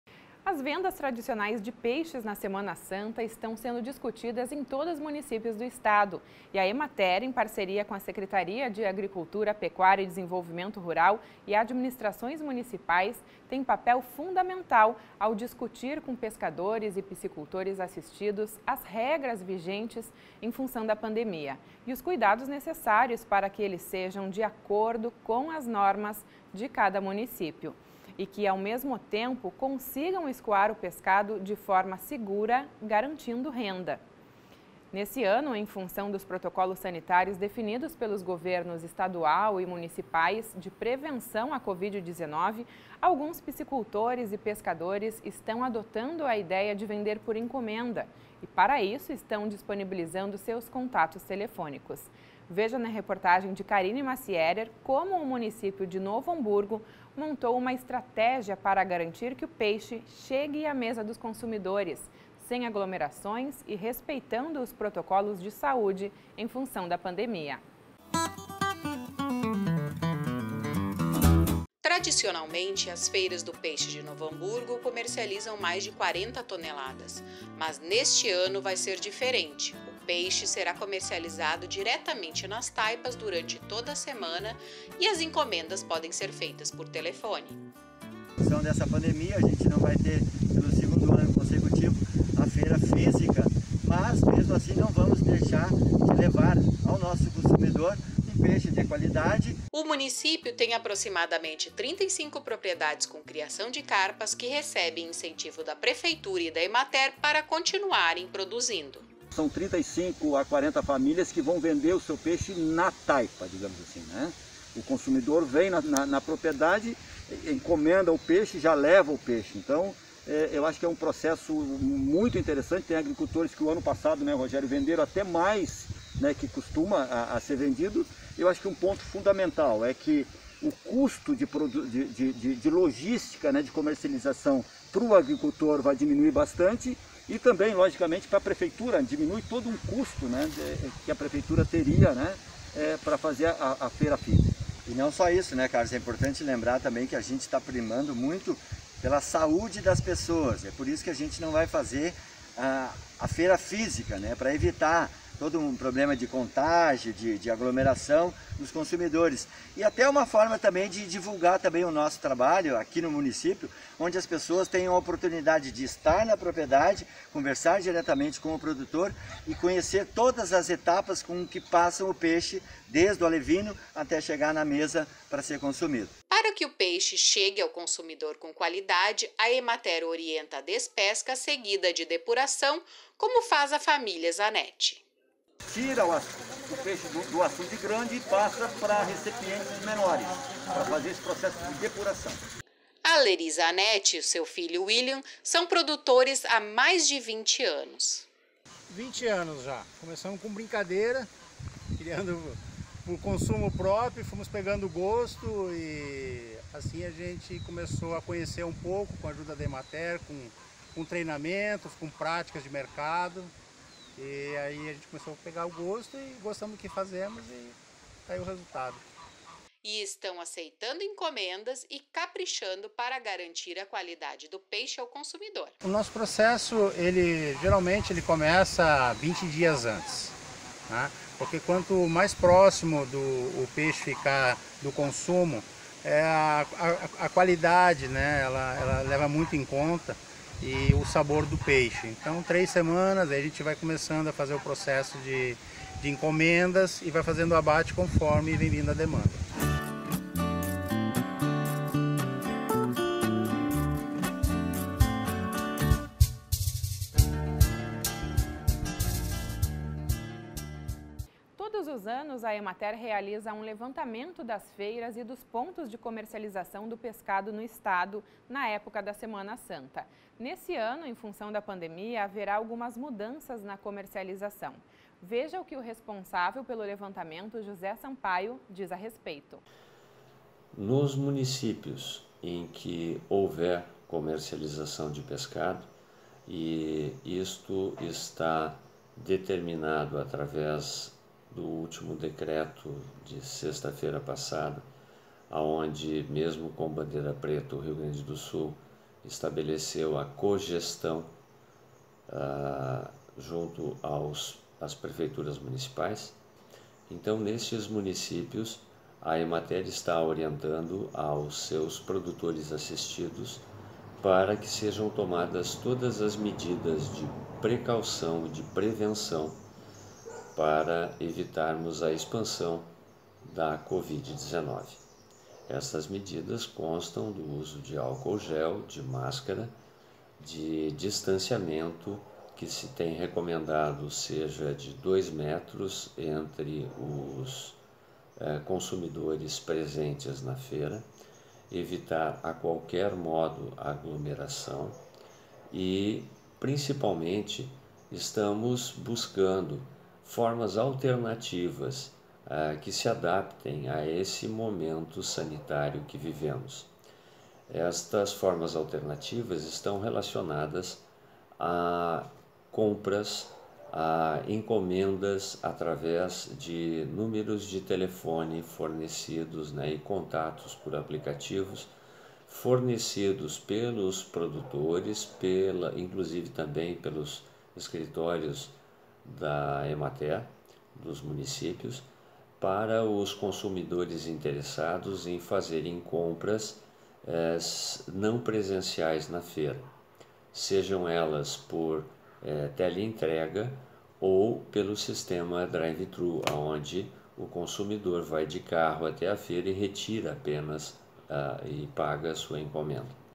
Venda de pescado na semana santa - Rio Grande Rural
As vendas tradicionais de peixes na Semana Santa estão sendo discutidas em todos os municípios do Estado e a Emater/RS-Ascar, em parceria com a Secretaria de Agricultura, Pecuária e Desenvolvimento Rural (Seapdr) e administrações municipais, tem papel fundamental ao discutir com pescadores e piscicultores assistidos as regras vigentes em função da pandemia e os cuidados necessários para que eles estejam de acordo com as normas de cada município. E que ao mesmo tempo consigam escoar o pescado de forma segura, garantindo renda.

Neste ano, em função dos protocolos sanitários definidos pelos governos estadual e municipais de prevenção à Covid-19 alguns piscicultores e pescadores estão adotando a ideia de vender por encomenda e para isto estão disponibilizando seus contatos telefônicos. Veja na reportagem de Carine Massierer, como o município de Novo Hamburgo montou uma estratégia para garantir que o peixe chegue a mesa dos consumidores, sem aglomerações e respeitando os protocolos de saúde em função da pandemia.